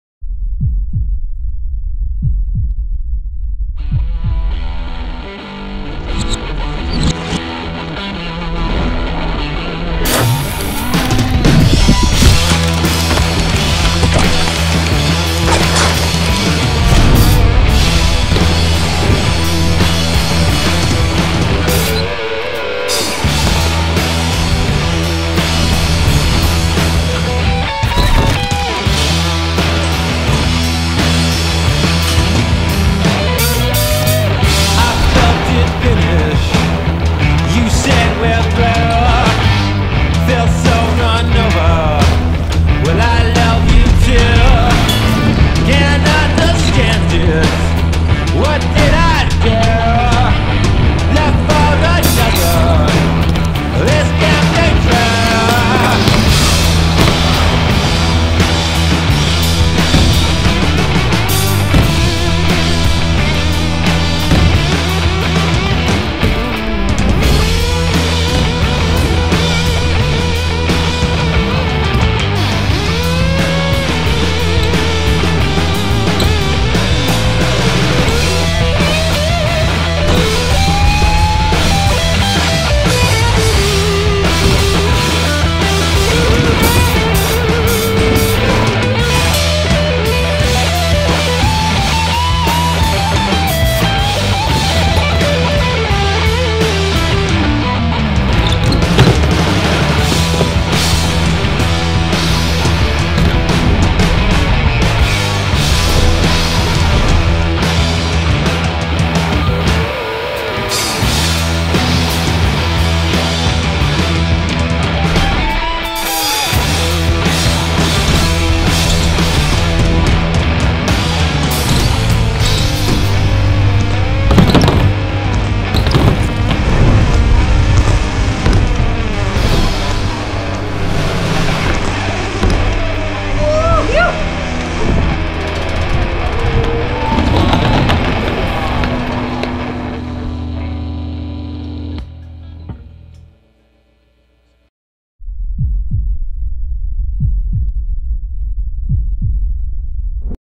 Dream Yard 4 ft. Pat Casey
The best backyard on the planet is back. Pat Casey drops in for the fourth installment of his Dreamyard series. What started in 2013 as a “basic” backyard ramp has grown in stages over the years: a massive set of trails were added in 2015 the ramp was boxed in and nearly doubled in size in 2017 and now, the ramps have evolved yet again. At nearly 10,000 square feet, the new additions include a truly daunting line that wraps around the entire structure—and an open loop. The one and only Dream Yard is bigger and better than ever, so watch as Pat breaks in the latest additions as only he can…